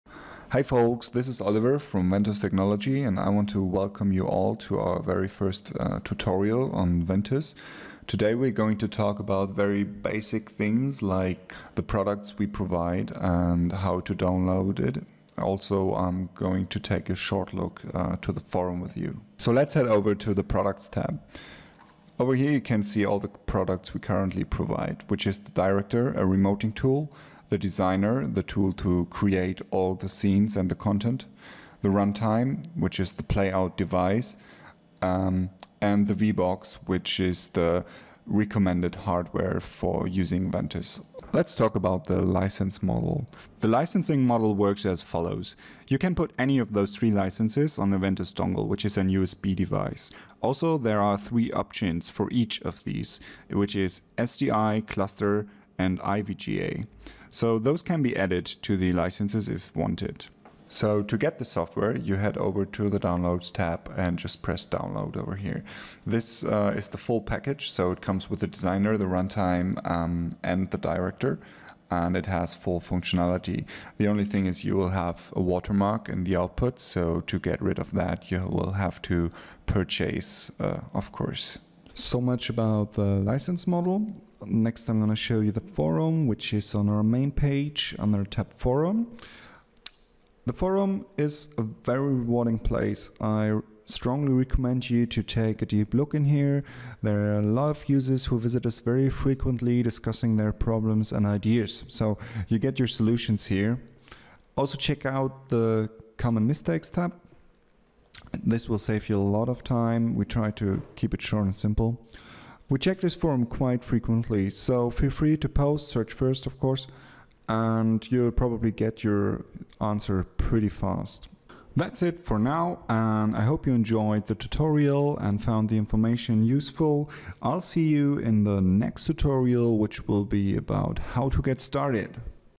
Ventuz Tutorial 01 - Introduction
In our very first tutorial we introduce you to the Ventuz product family, explain our license model and show you where to find crucial information.

This is one of a series of tutorials on how to learn and use Ventuz.  You can download the personal learning edition for free to follow along with what you learn in the videos.

Once you become familiar with Ventuz you can use if for interactive 3D presentations,  real-time events, projection mapping, display walls and digital signage, broadcast on-air graphics and more. The only limitation is your imagination.